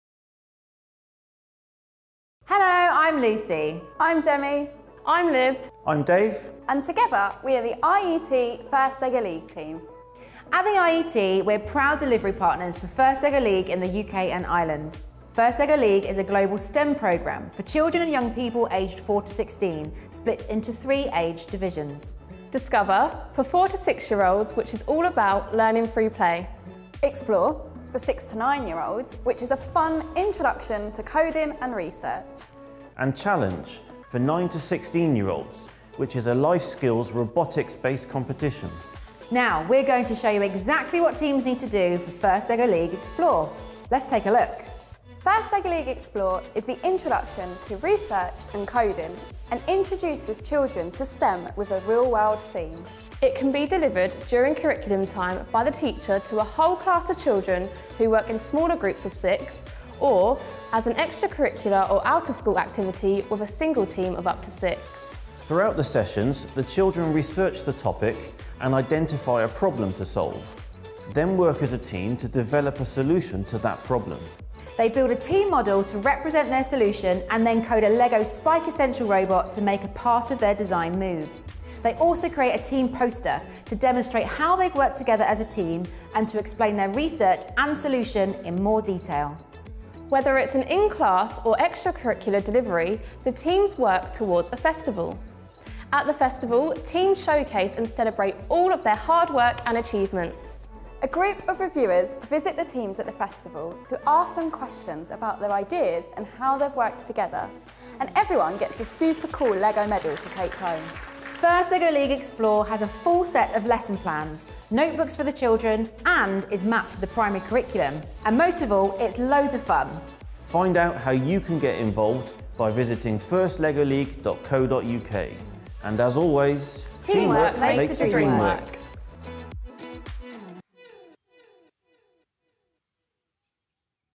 What is FIRST LEGO League Explore?
FIRST LEGO League Explore is a global STEM (that’s science technology engineering and maths) programme for children and young people aged 6-9. The IET are proud to be the delivery partners for FIRST LEGO League in the UK and Ireland, because FIRST LEGO League is the perfect programme to start young people thinking about engineering as a potential career path.

FIRST LEGO League Explore introduces children to coding and research, linked to real-world contexts. Alongside developing STEM skills, it’s a life-skills programme that develops skills such as teamwork and persistence. Find out more about FIRST LEGO League Explore and how you can get involved!